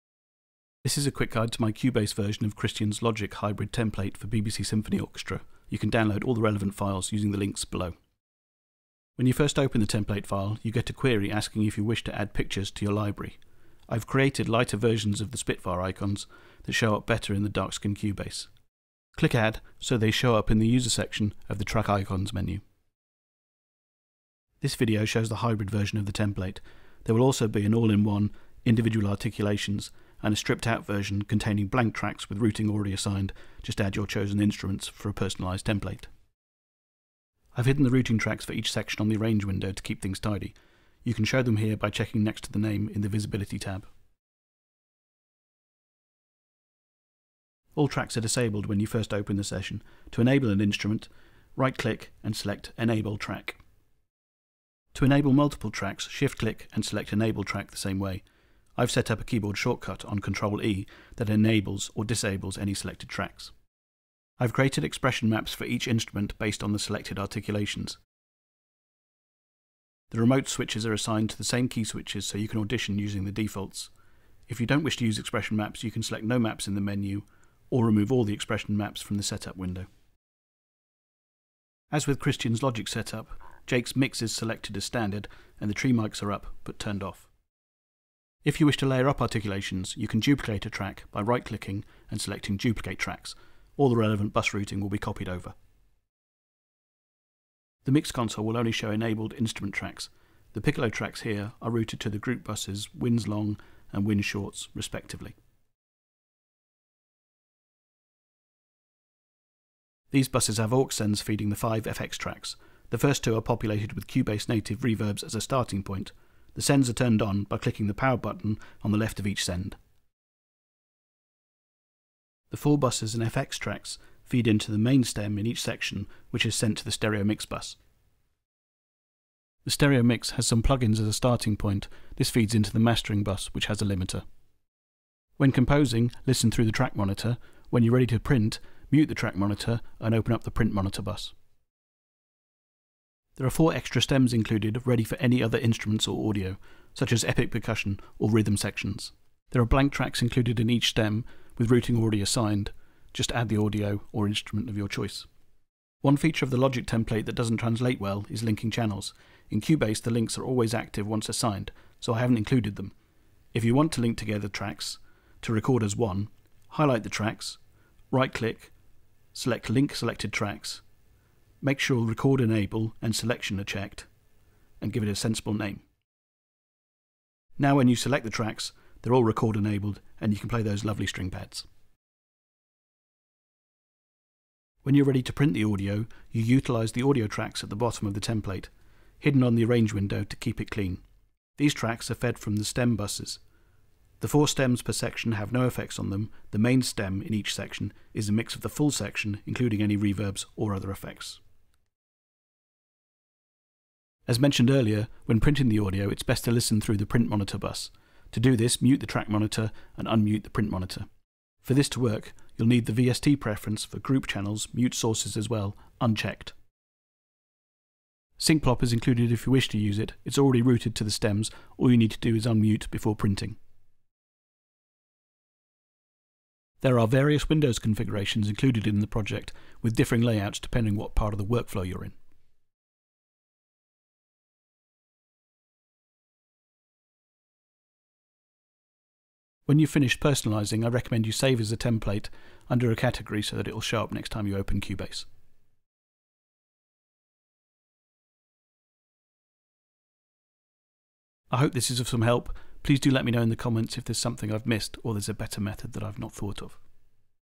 Spitfire BBCSO Hybrid Template - Cubase Version
A quick tutorial of the layout and routing of my Cubase 10 version of Christian Henson's hybrid template for Spitfire's BBCSO

Since the update of BBCSO to offer Discover and Core, new templates have been created. I've updated the Pro version, and created new templates inline with the new one's created by Christian and Jake. All can be found at Spitfire's The